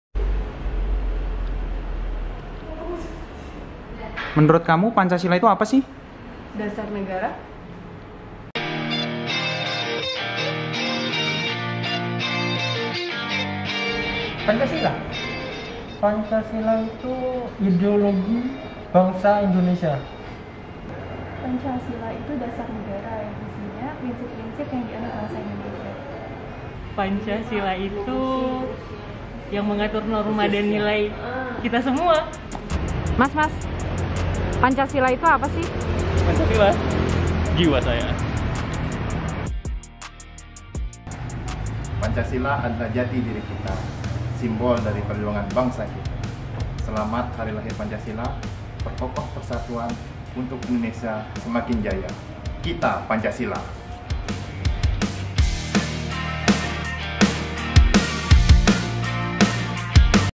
7. Hari Lahir Pancasila - Novia Indriani - Cabang Bangka Belitung
Dalam rangka memperingati 76 tahun Pancasila, Ayo ekspresikan rasa cinta tanah air-mu pada peringatan Hari Lahir Pancasila 1 Juni 2021 mendatang. Untuk menjaga pentingnya Pancasila sebagai dasar ideologi bangsa melalui video singkat
Kali ini Divisi Human Capital Jasa Raharja berkolaborasi dengan Spirit of Millennials mengadakan lomba dengan tema kebangsaan dan mengajak teman – teman sekalian untu berpartisipasi dalam “Lomba Video Hari Lahir Pancasila dengan tema KITA PANCASILA”.